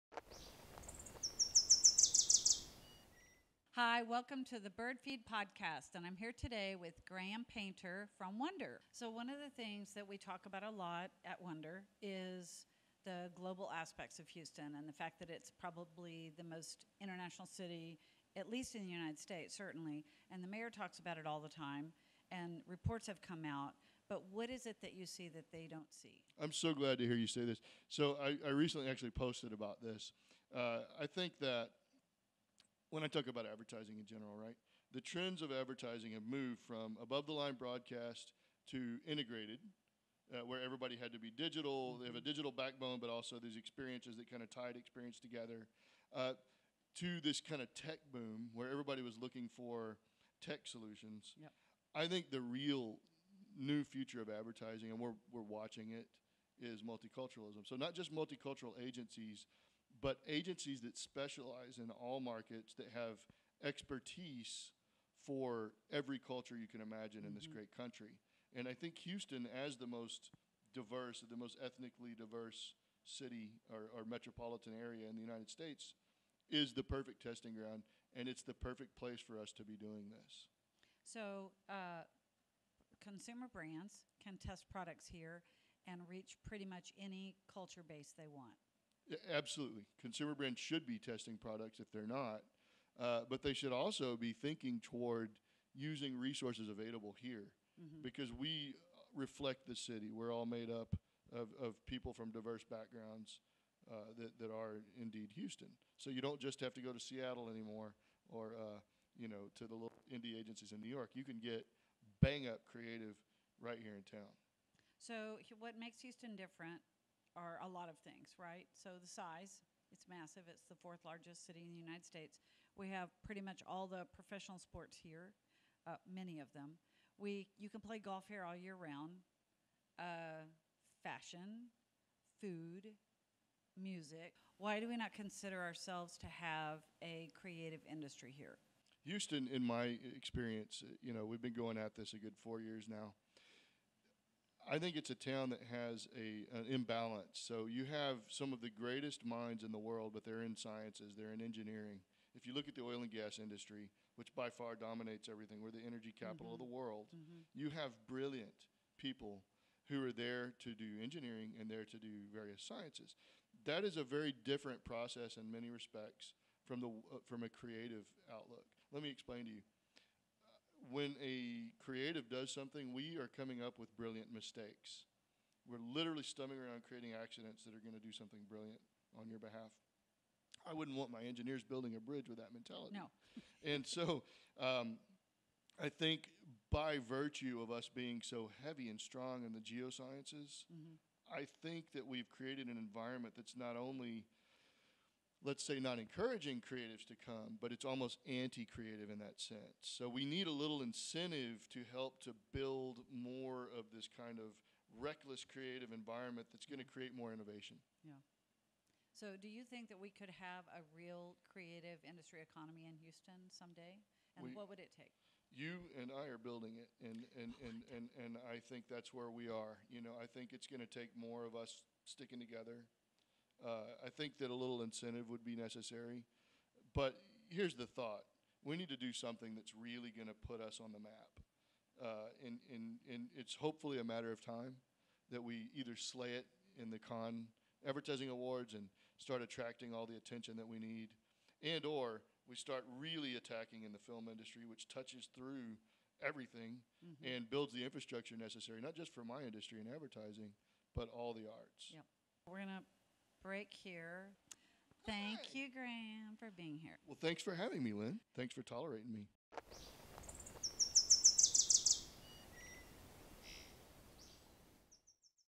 Building the Houston Creative Economy with Guest Graham Painter, COO of WOND3R
"I grew up in Houston. I went to the high school for the performing and visual arts, wanted to be a jazz musician. But no matter how much I practiced, no matter how hard I shed, [my trumpet] wasn't going to give me that same love back. So, I decided to get rich and stop majoring in music and major in philosophy. Haha! Right."

Graham Painter takes a few sharp turns to become a global brand creative director with several internationally renowned advertising agencies. But Houston wasn't ready for him. In this week's Bird Feed podcast, hear from this eclectic and fascinating anti-ad man who created his own opportunities when he founded WOND3R in EaDo.